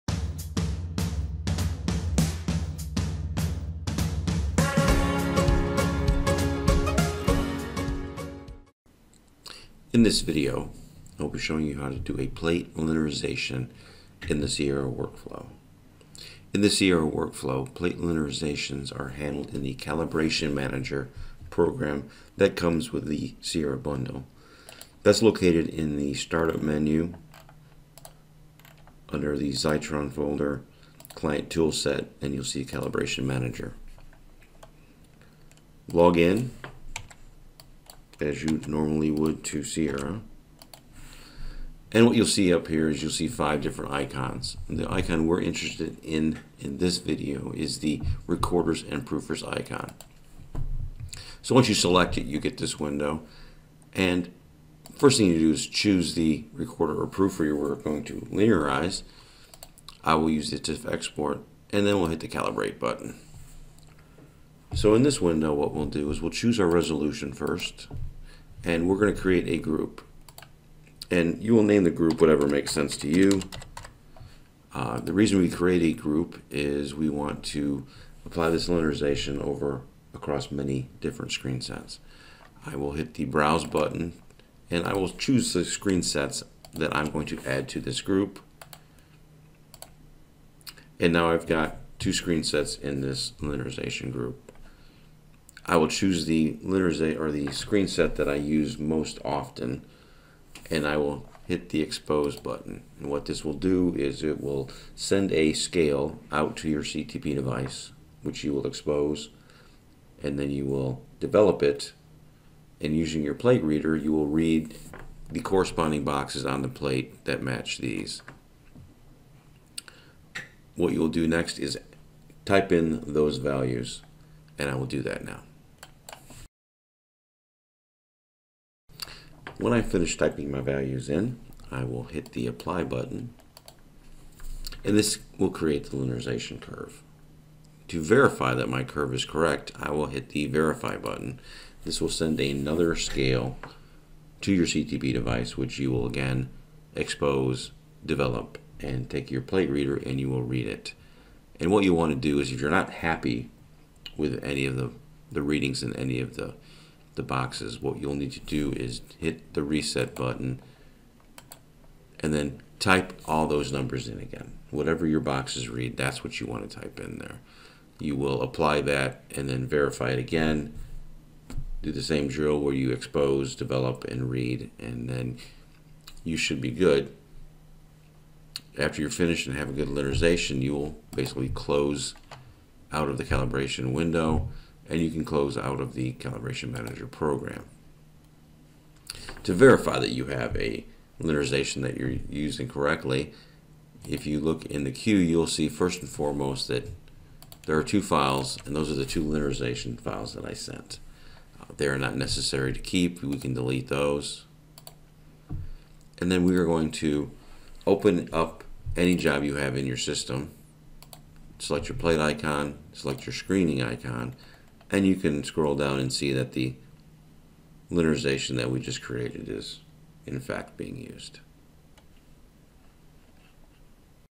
Sierra Plate Linearization
How to linearize plates in Sierra APPE workflow